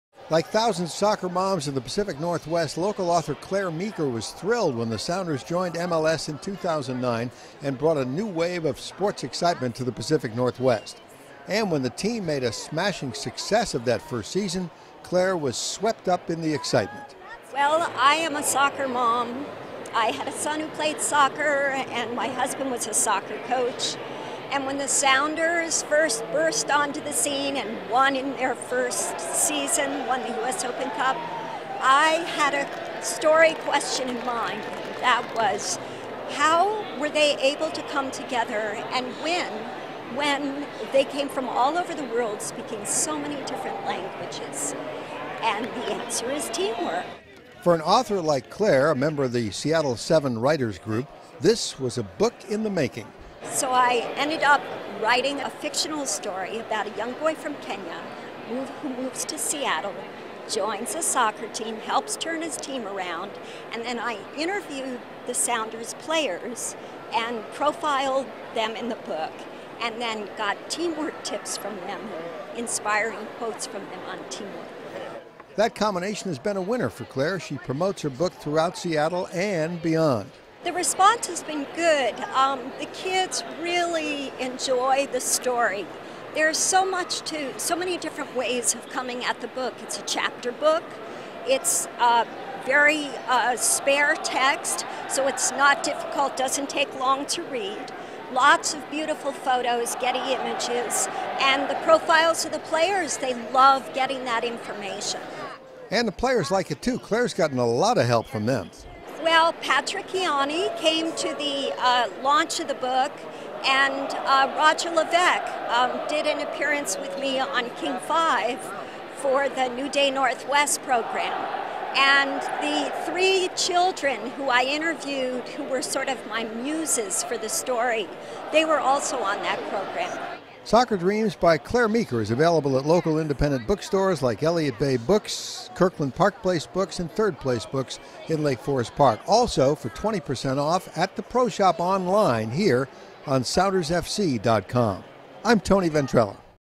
Tony Ventrella Interviews Clare Meeker about Soccer Dreams
An interview with author Clare Meeker